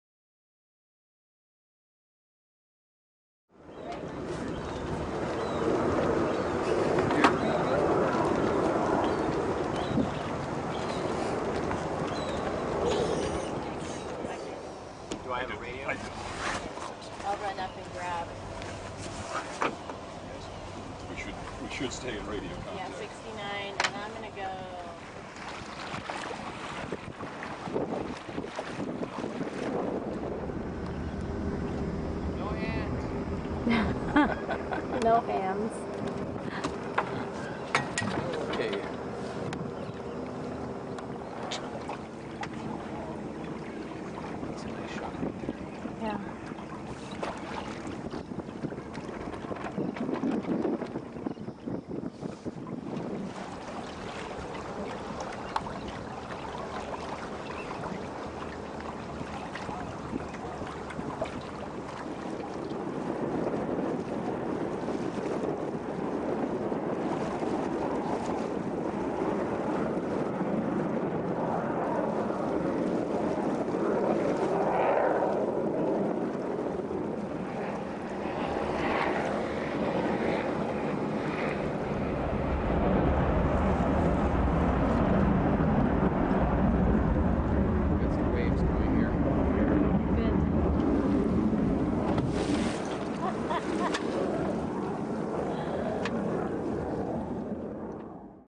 Ken Sailing August 15, 2010 HQ
This is video footage from the first time I went sailing in San Diego near shelter Island. I was in control of the boat using a joystick, similar to the one on my wheelchair. It was very fun and very relaxing under the warm sun and cool breeze.

The organization that made this possible is called Challenged Sailors, formerly Challenged America. This is a great program that takes people out with various disabilities sailing in the San Diego harbor.